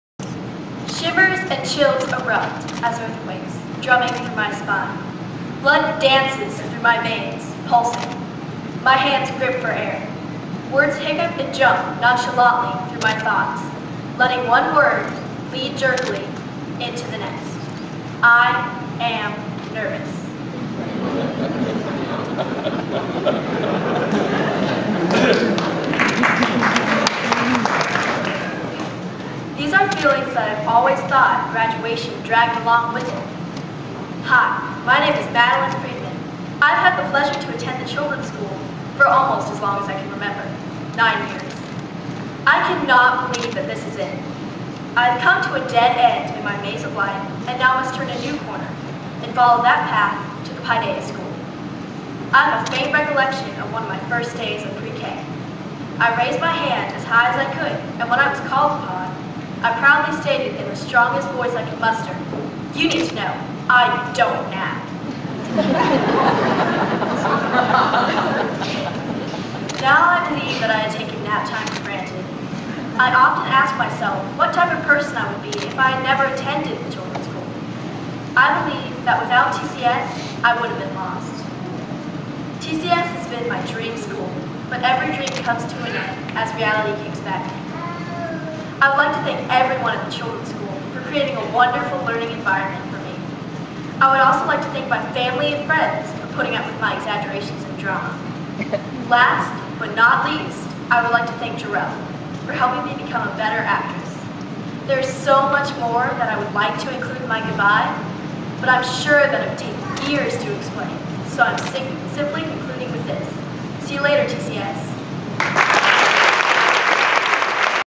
Maddi's graduation speech
Maddi starts her speech by a quote which turns out to be her own.